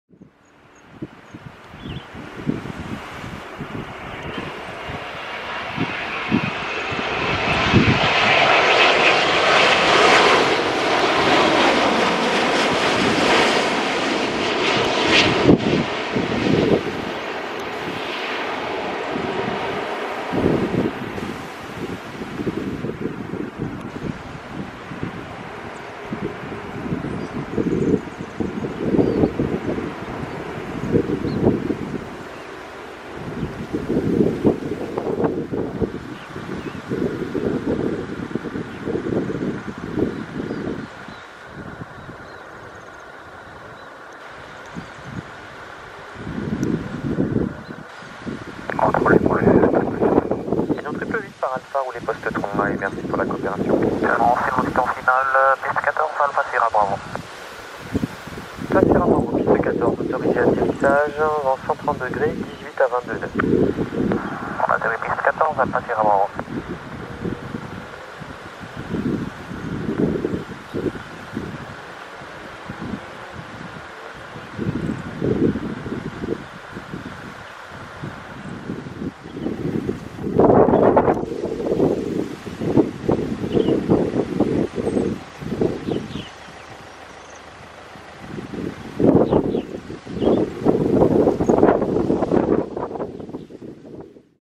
Air Austral, Bangkok - St Denis de la Réunion (RUN),Boeing 787-8
Arrivée de Bangkok (VTBS/BKK) ,vol REU 888